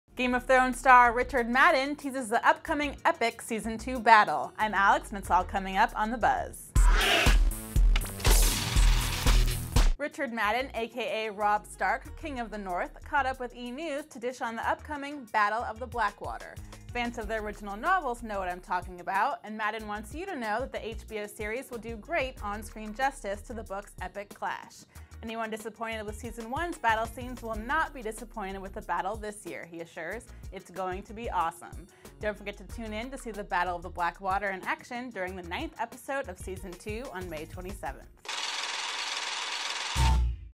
Game Of Thrones' Robb Stark Interview Scoop: Richard Madden Teases 'Battle Of Blackwater'
Game of Thrones star Richard Madden (aka Robb Stark, King of the North) dished on the upcoming epic 'Battle of the Blackwater.'

Transcription:
Game of Thrones star Richard Madden teases the upcoming epic season 2 battle. I'm Alex and it's all coming up on The Buzz.
Richard Madden aka Robb Stark, King of the North, caught up with E! News to dish on the upcoming 'Battle of the Blackwater.' Fans of the original novels know what I'm talking about, and Madden wants you to know that the HBO series will do great on-screen justice to the book's epic clash. Anyone disappointed with season 1's battle scenes will "not be disappointed with the battle this year," he assures. "It's going to be awesome." Don't forget to tune in to see the Battle of the Blackwater' in action during the ninth episode of season 2, on May 27th.